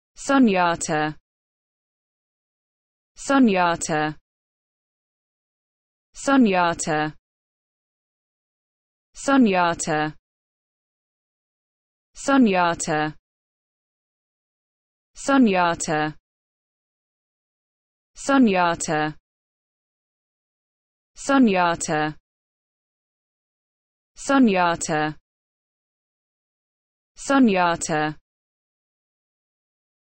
How To Pronounce Sunyata
This video shows you how to pronounce Sunyata in English.